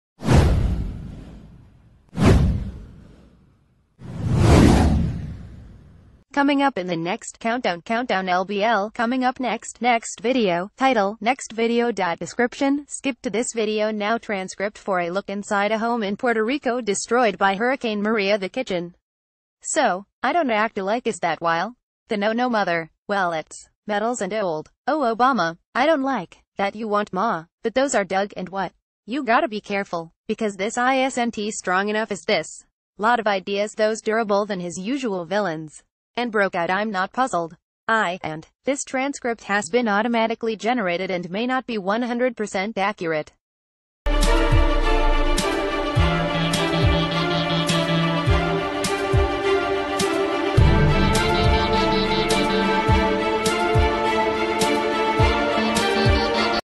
Breaking News | Video: a look inside a home in puerto rico destroyed by hurricane maria
Video: a look inside a home in puerto rico destroyed by hurricane maria

Coming up in the next {{countdown}} {{countdownlbl}} Coming up next {{nextVideo.title}} {{nextVideo.